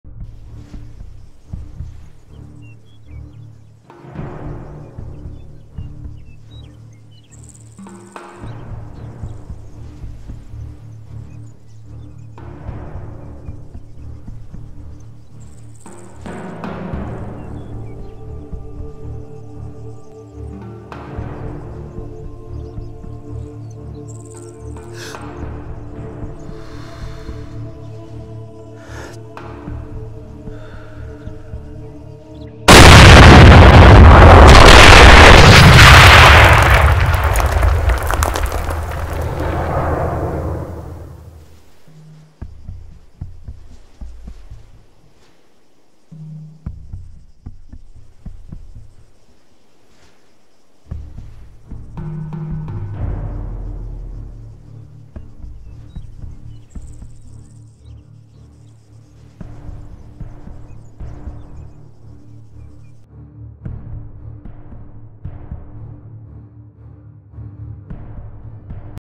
Sniper 1 shot 5 kills Target had explosive belt around
Scharfschütze 1 Schuss 5 Tote Ziel hatte Sprengstoffgürtel um